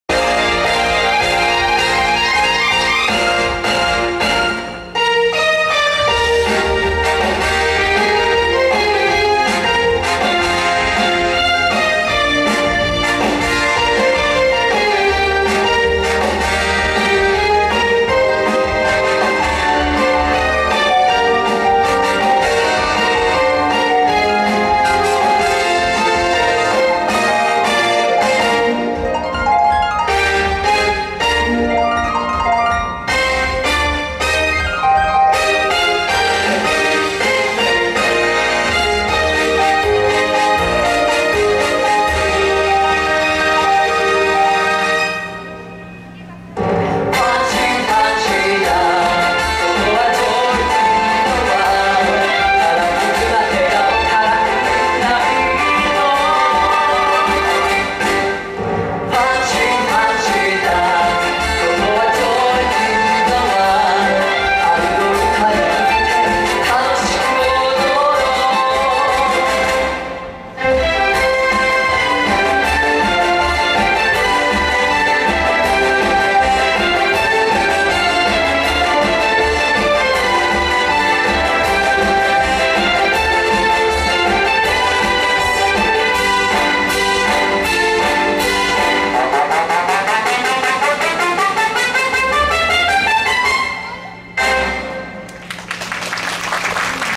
20230304 チームハピネス 泉美匠さん 推しカメ
歌劇(チーム奏)『Fancy Spring Review Showハピネス ～春の歌にのせて～』 ハウステンボス歌劇団　チームハピネス